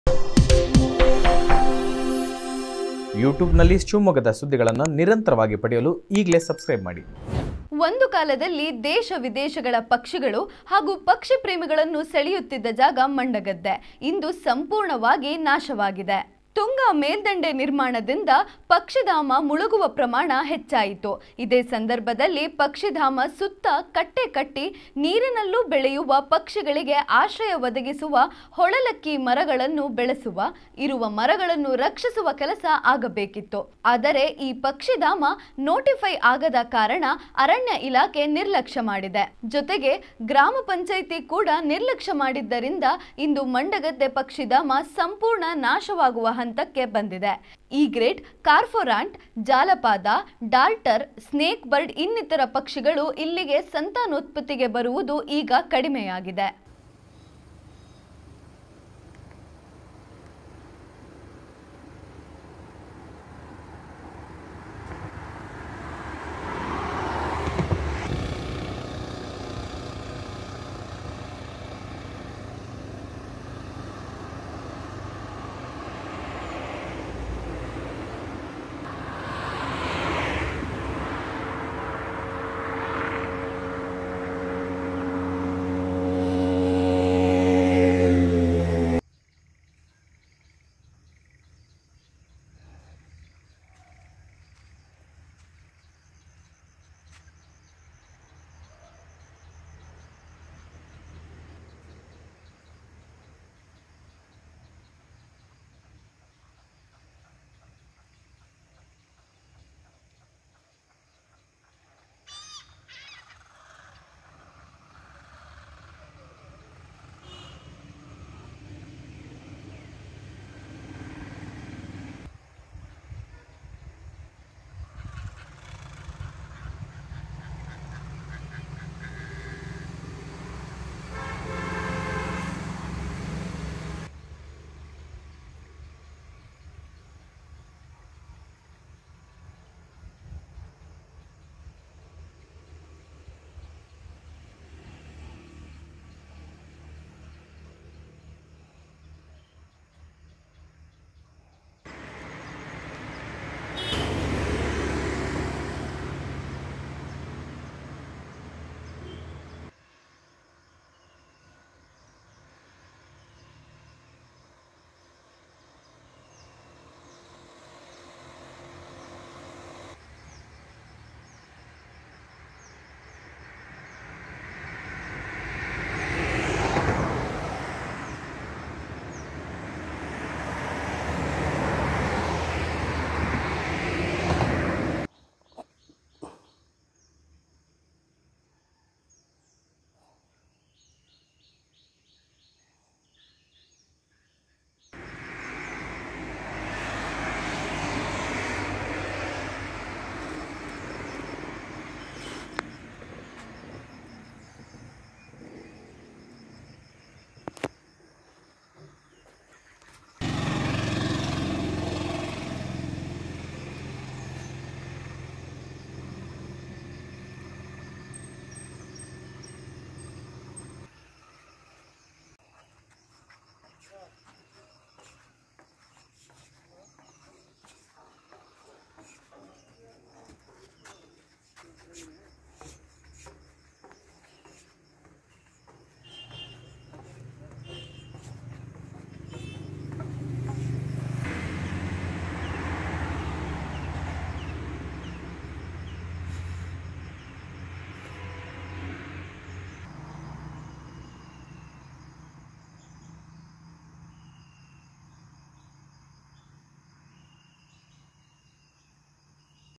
ಮಂಡಗದ್ದೆ ಪಕ್ಷಿಧಾಮ- ಇನ್ನು ನೆನಪು ಮಾತ್ರ Mandagadde Bird Sanctuary - just a memorial now
ಒಂದು ಕಾಲದಲ್ಲಿ ದೇಶ-ವಿದೇಶಗಳ ಪಕ್ಷಿಗಳನ್ನು ಹಾಗೂ ಪಕ್ಷಿ ಪ್ರೇಮಿಗಳನ್ನು ಸೆಳೆಯುತ್ತಿದ್ದ ಮಂಡಗದ್ದೆ ಪಕ್ಷಿಧಾಮ ಇಂದು ಸಂಪೂರ್ಣ ನಾಶವಾಗಿದೆ. ತುಂಗಾ ಮೇಲ್ದಂಡೆ ನಿರ್ಮಾಣದಿಂದ ಪಕ್ಷಿಧಾಮ ಮುಳುಗುವ ಪ್ರಮಾಣ ಹೆಚ್ಚಾಯಿತು. ಇದೇ ಸಂದರ್ಭದಲ್ಲಿ ಪಕ್ಷಿಧಾಮದ ಸುತ್ತ ಕಟ್ಟೆ ಕಟ್ಟೆ, ನೀರಿನಲ್ಲೂ ಬೆಳೆಯುವ, ಪಕ್ಷಿಗಳಿಗೆ ಆಶ್ರಯ ಒದಗಿಸುವ ಹೊಳೆಲಕ್ಕಿ ಮರಗಳನ್ನು ಬೆಳೆಸುವ, ಇರುವ ಮರಗಳನ್ನು ರಕ್ಷಿಸುವ ಕೆಲಸ ಆಗಬೇಕಿತ್ತು. ಆದರೆ, ಈ ಪಕ್ಷಿಧಾಮ ನೋಟಿಫೈ ಆಗದ ಕಾರಣ ಅರಣ್ಯ ಇಲಾಖೆ ನಿರ್ಲಕ್ಷ್ಯ ಮಾಡಿದೆ. ಜೊತೆಗೆ ಗ್ರಾಮ ಪಂಚಾಯ್ತಿ ಕೂಡಾ ನಿರ್ಲಕ್ಷ್ಯ ಮಾಡಿದ್ದರಿಂದ ಇಂದು ಮಂಡಗದ್ದೆ ಪಕ್ಷಿಧಾಮ ಸಂಪೂರ್ಣ ನಾಶವಾಗುವ ಹಂತಕ್ಕೆ ಬಂದಿದೆ. ಈಗ್ರೇಟ್, ಕಾರ್ವೊರಾಂಟ್, ಜಾಲಪಾದ, ಡಾರ್ಟರ್, ಸ್ನೇಕ್ ಬರ್ಡ್ ಇನ್ನಿತರ ಪಕ್ಷಿಗಳು ಇಲ್ಲಿಗೆ ಸಂತಾನೋತ್ಪತ್ತಿಗೆ ಬರುವುದು ಈಗ ಕಡಿಮೆ ಆಗಿದೆ.